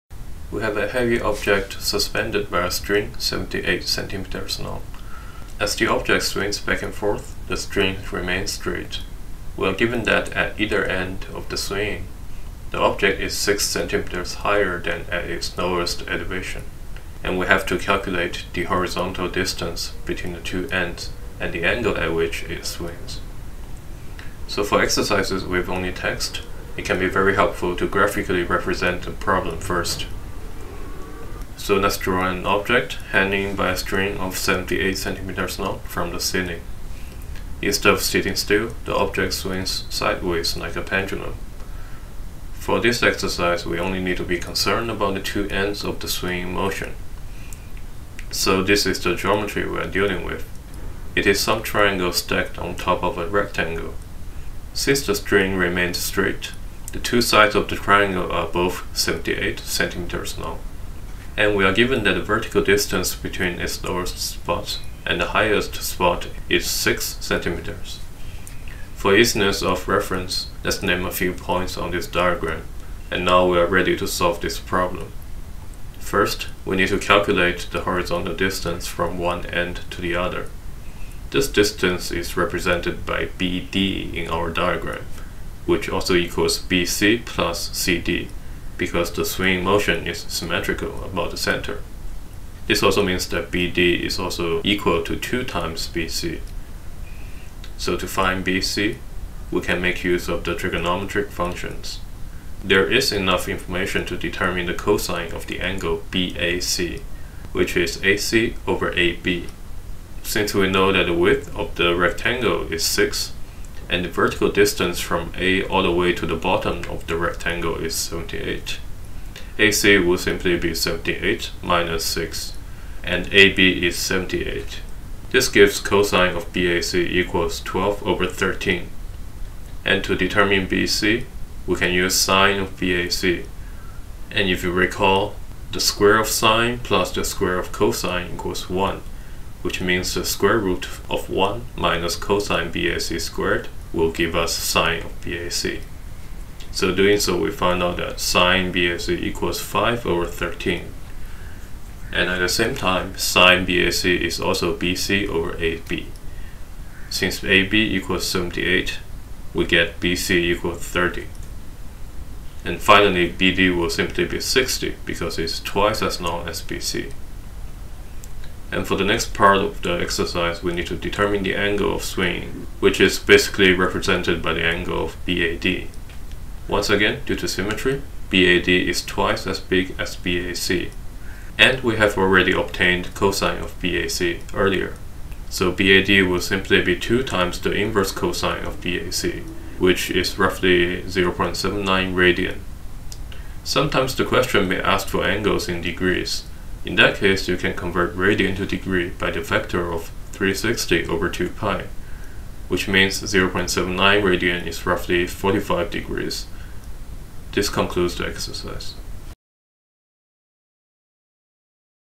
A-Math: Basic application of trigonometry in swinging motion
An object is suspended at one end of a string. Given the length of the string and height difference of the object during swinging, this exercise asks for the horizontal swinging distance and swinging angle.